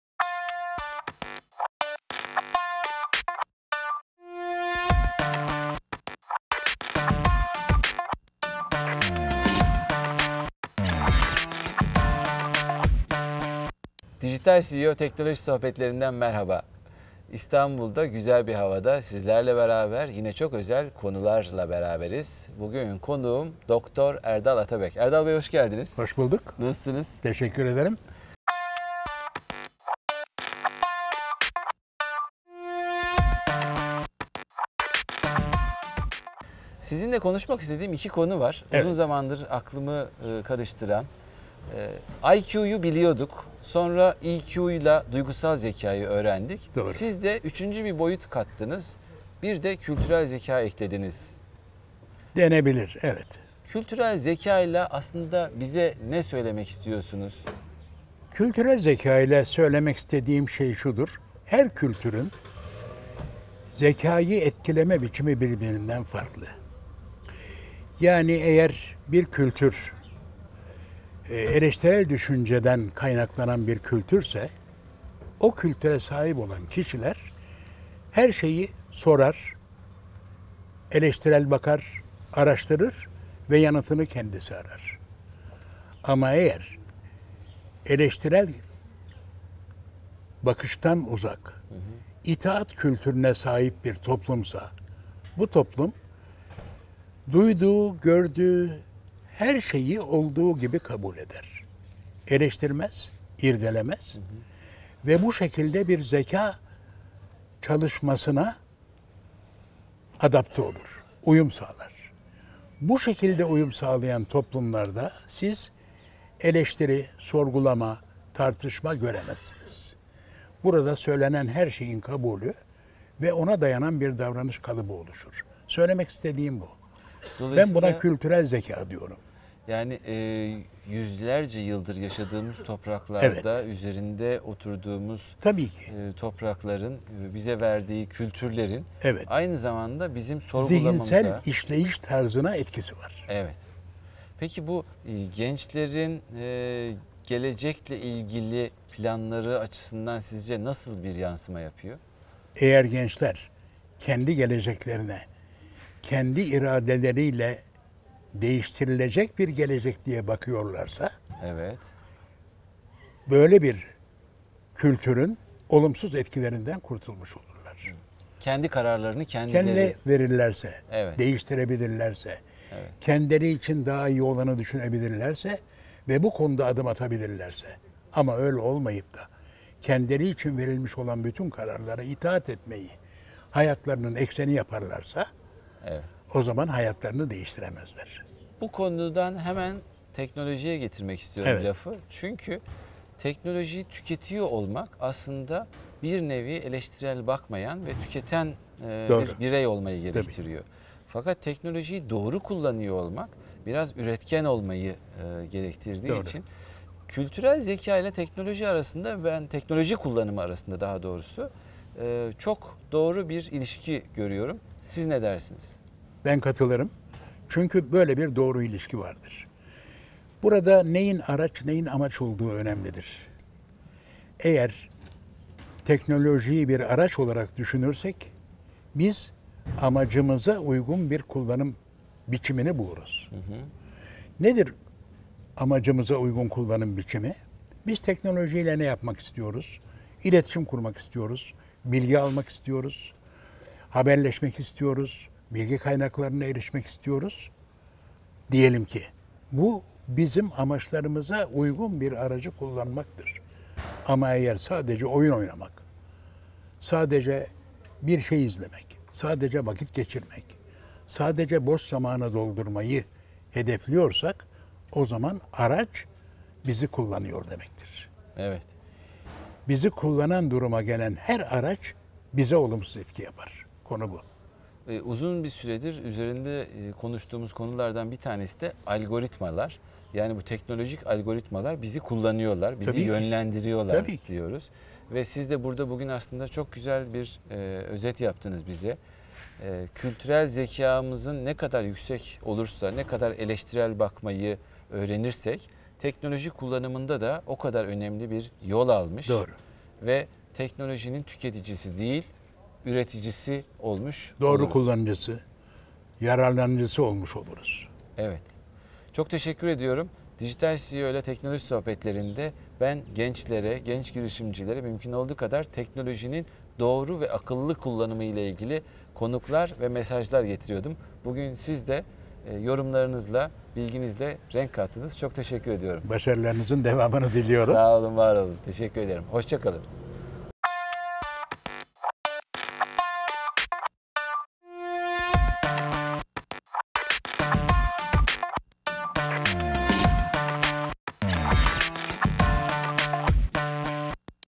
Kültürel Zeka - Dr. Erdal Atabek / Dijital CEO ile Teknoloji Sohbetleri #52
Dijital CEO İle Teknoloji Sohbetleri'nin 52'inci programında konuğum Dr. Erdal Atabek oldu.

'Kültürel zeka' konusunu ele aldığımız bu keyifli sohbette özellikle ‘kültürel zekanın’ teknoloji ilişkisine de değindik.

İyi seyirler

Dr. Erdal Atabek'in köşe yazılarına ulaşmak için